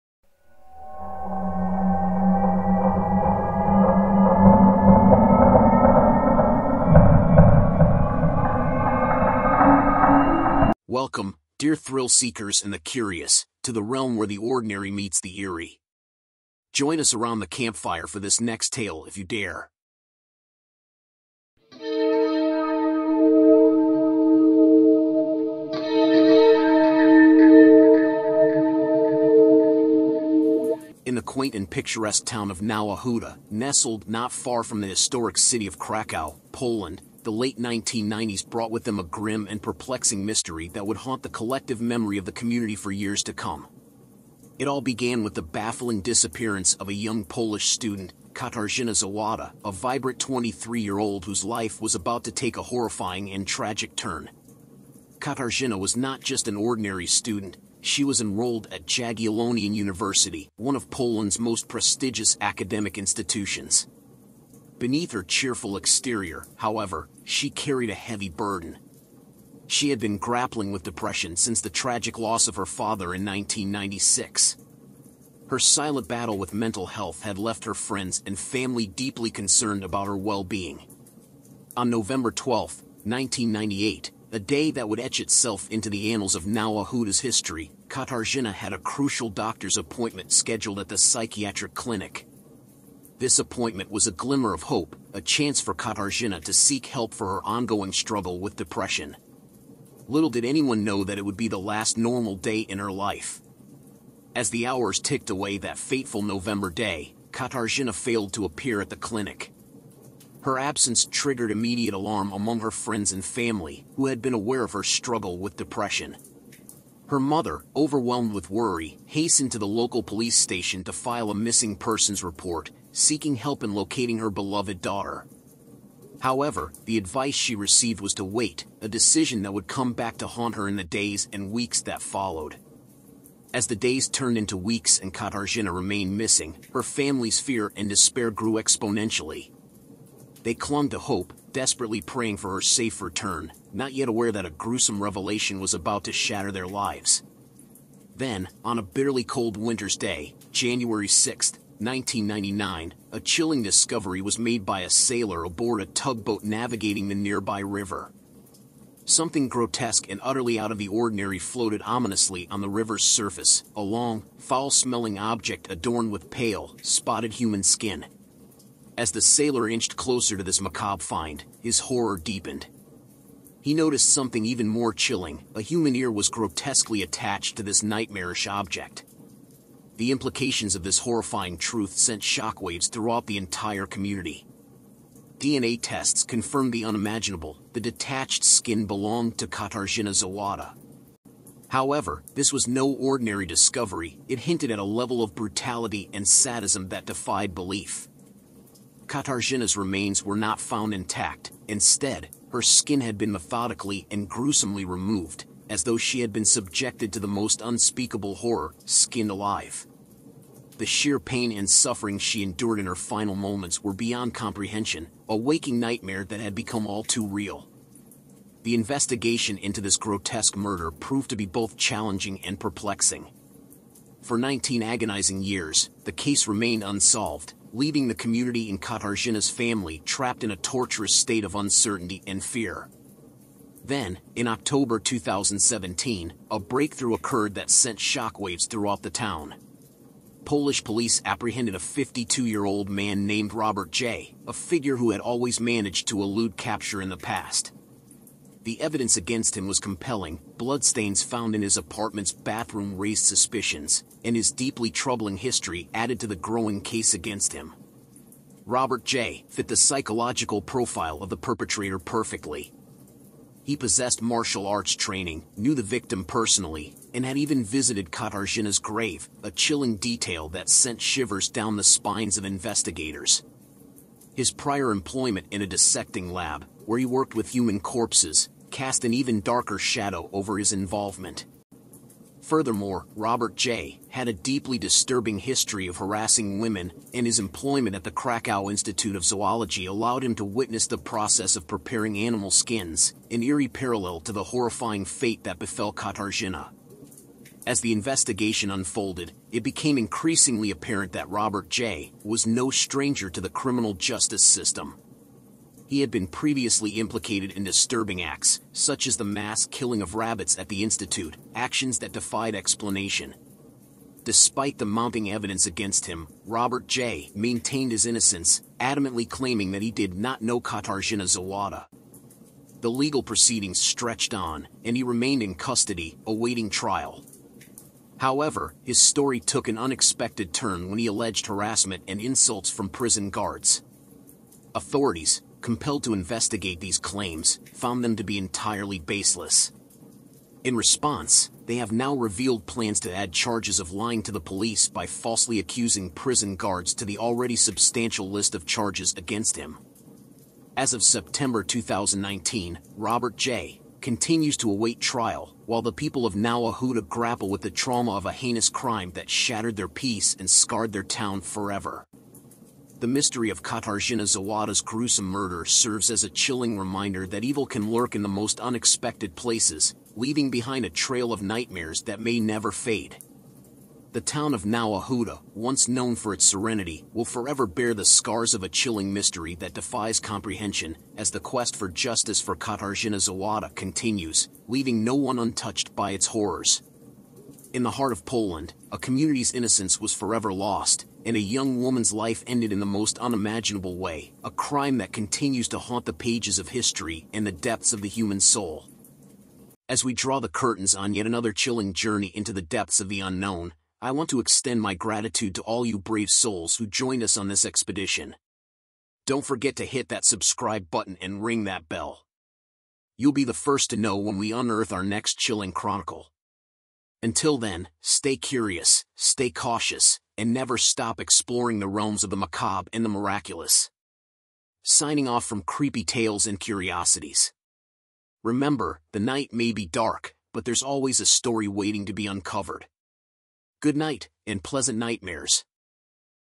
The Disturbing Skin Case: Katarzyna Zowada
Explore the spine-chilling mystery of Katarzyna Zowada's skin murder case in our latest video. Join us as we delve deep into this disturbing and perplexing unsolved crime that has left investigators baffled for years. Discover the gruesome details of Katarzyna's tragic fate, from her mysterious disappearance to the horrifying discovery of her skinned remains.
In this video, we'll unravel the enigmatic events surrounding Katarzyna's case, including shocking revelations about her alleged killer, the investigation's twists and turns, and the eerie connection to martial arts and dissecting labs. Join us on this eerie journey into the heart of darkness, where truth is stranger than fiction.
Leave your thoughts and theories in the comments below, and let's piece together the puzzle of the Katarzyna Zowada skin murder case together.